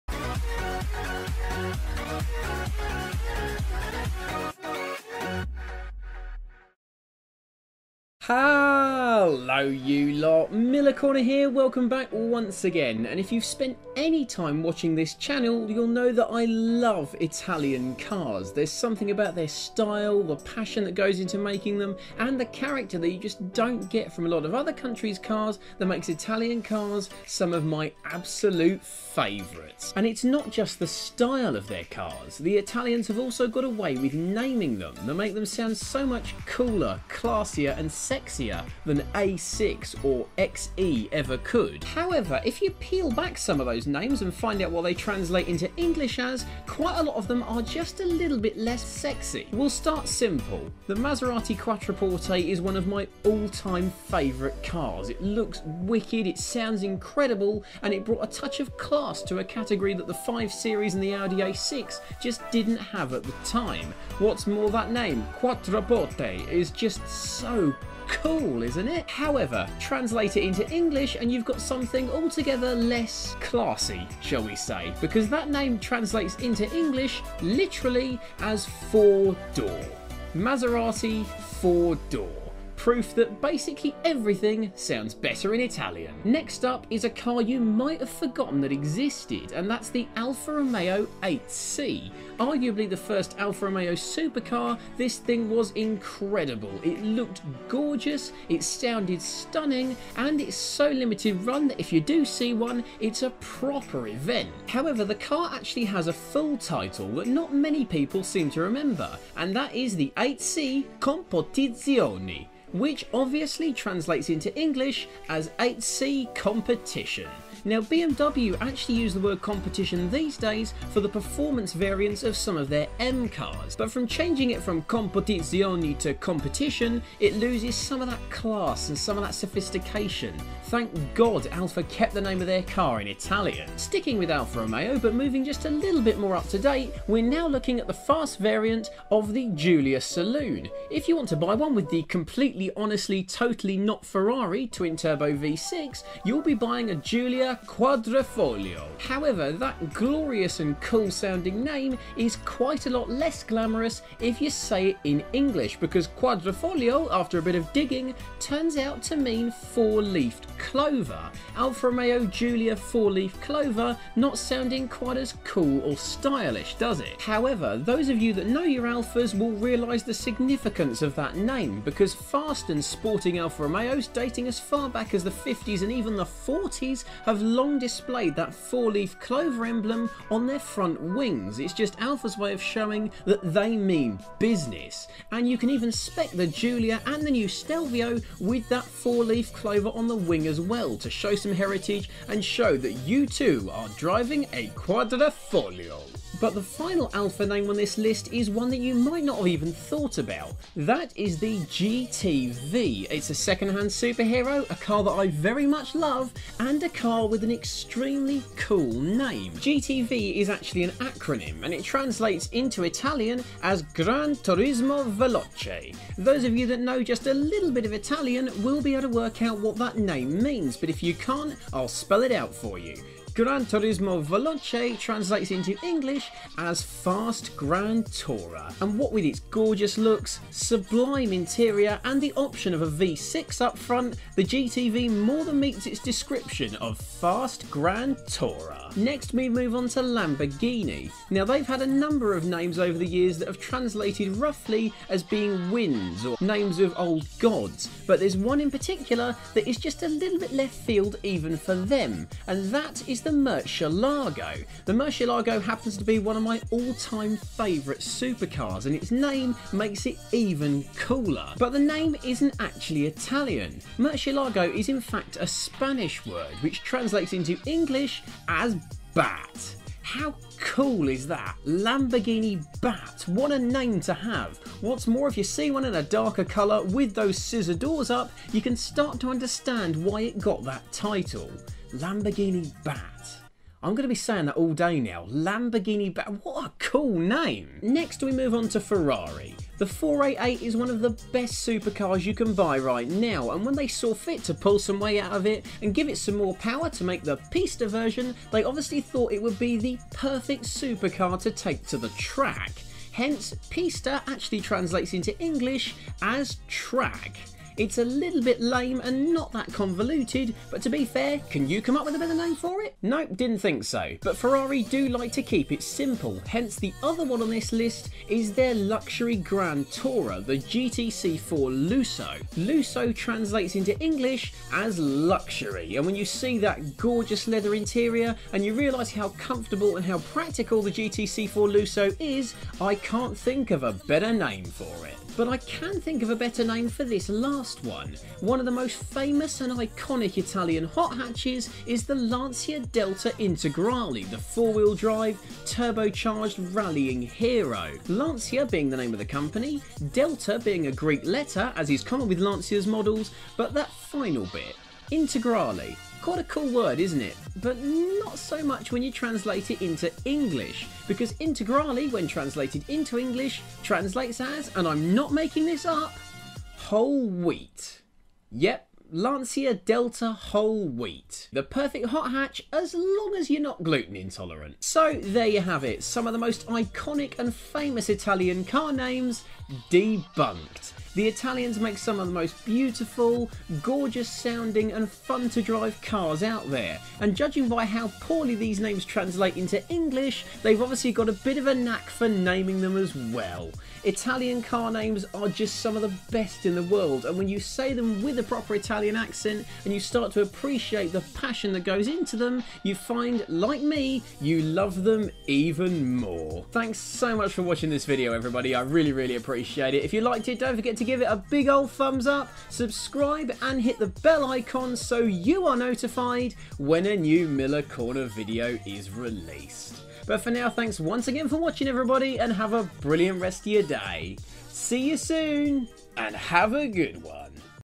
Italian Car Names REAL Meanings!
Italian cars are amongst the sexiest and most evocative around, not least because of their incredible names. HOWEVER, peel some of those names back, find out their meanings in English and they do lose a *little* bit of class and style…

All content on this channel is - unless otherwise stated - my opinion and any offense caused is severely apologised for. All rights respected.